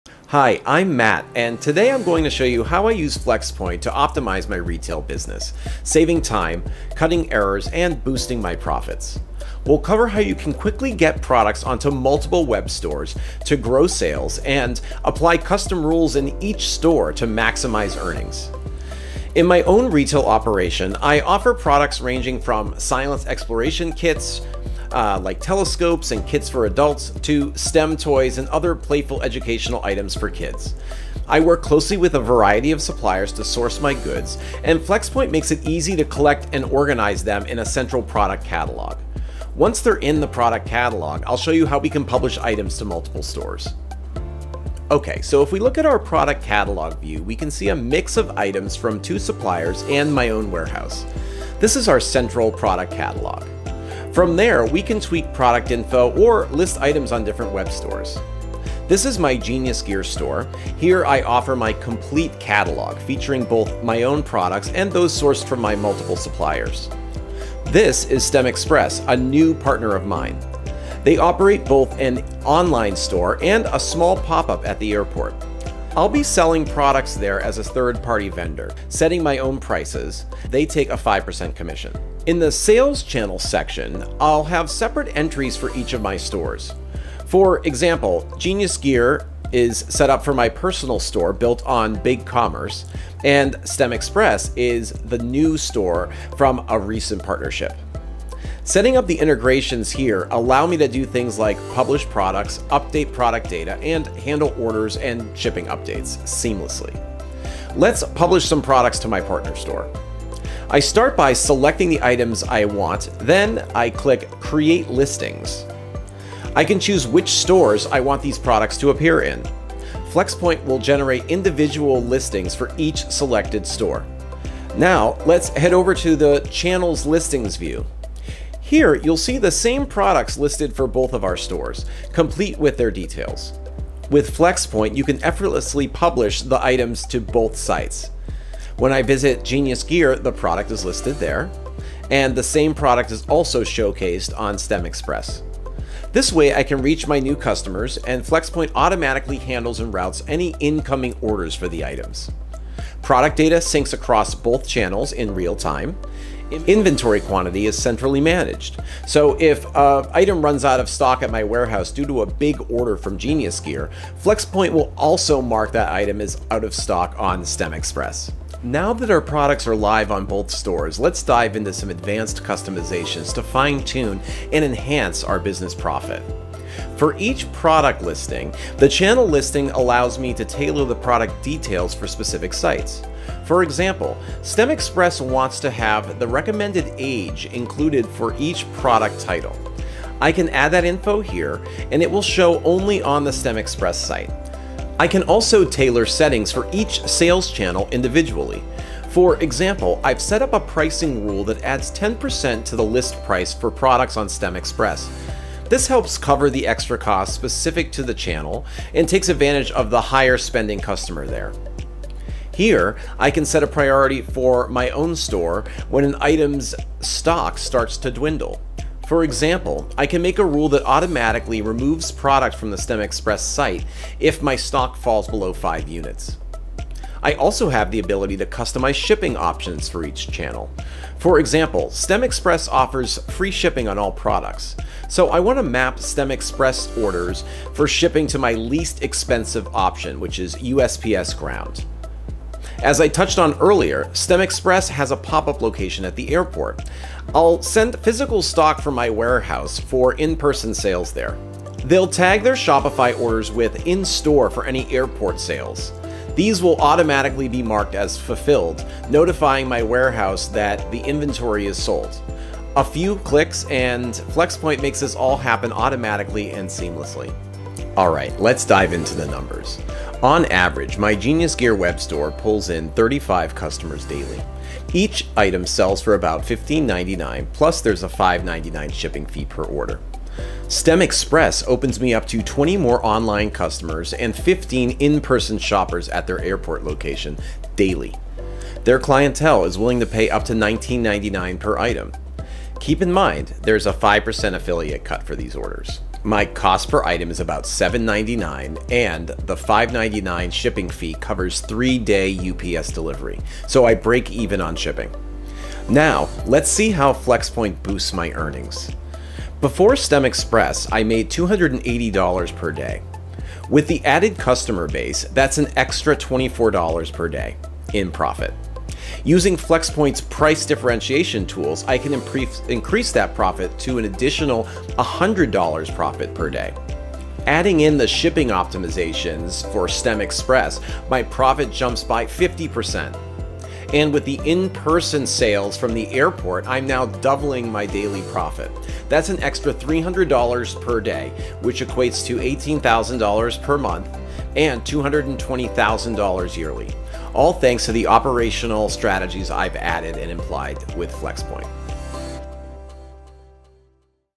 Episode 2: How To Handle Dropshipping From Multiple Suppliers [Shopify and BigCommerce]
Welcome to the 2nd episode of our 5 Part Video Series on "Real Dropshipping Automation ROI Use Cases" hosted by Matt Bonaccorso.

Matt Bonaccorso is an ecommerce consultant who runs his own ecommerce businesses and knows how to get the most out of Flxpoint.

Each video has a ROI breakdown & calculation you must watch!

In Episode 2, Matt continues to showcase how Flxpoint effortlessly integrates and synchronizes inventory, orders, and product data across multiple sales channels.

He demonstrates the platform's ability to manage listings, track inventory in real-time, and centralize order processing from various marketplaces.

This video provides a detailed walk through on using Flxpoint to enhance retail business operations. It begins with an overview and then delves into the central product catalog's capabilities, demonstrating how it simplifies product management across multiple suppliers.

Discover how easy it is to list products on partner web stores using Flxpoint. Plus, explore advanced customizations for different sales channels, allowing for tailored product details and pricing strategies.

In addition, witness how Flxpoint can significantly boost order profitability with real-life examples of profit growth and operational efficiency in a retail setting.

If you're looking to streamline your multi-channel retail operations, this video is a must-watch.

00:00 Introduction
00:44 Overview of the central product catalog and its benefits
01:47 Demonstrating how to publish products to partner stores using Flxpoint
02:48 Advanced customizations for channel-specific settings
04:28 Maximizing Profitability (ROI Calculation)